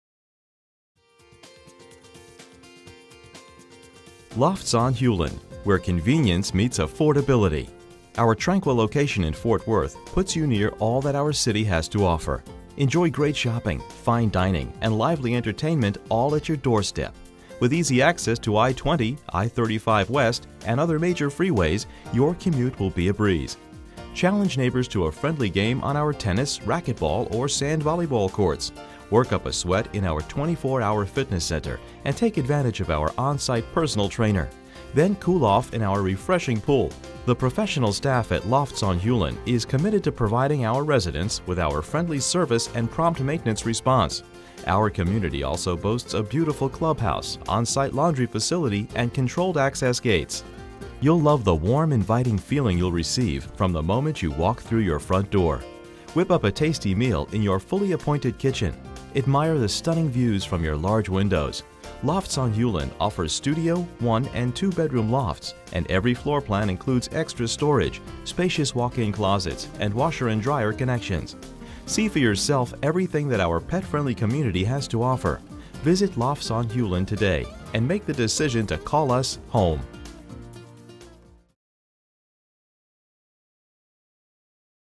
on Hulen Apartments in Fort Worth, TX - ForRent.com
on Hulen Apartments for rent in Fort Worth, TX on ForRent.com: Availability, pricing and special promotions subject to change daily. Located in Fort Worth, minutes from Hulen Mall and Benbrook Lake, Lofts at Hulen offers relaxed living. Free yourself from the burdens of home maintenance in this collection of one and two bedroom apartment homes with a variety of unique floorplans to fit every lifestyle. Our homes feature fully equipped kitchens, wood burning fireplaces and walk in closets. Residents enjoy a swimming pool, racquetball court, friendly staff and sand volleyball courts. Parks, schools and major employers shape the community, while established shopping, entertainment and recreation areas are nearby! Lease today! - 6500 Hulen Bend Blvd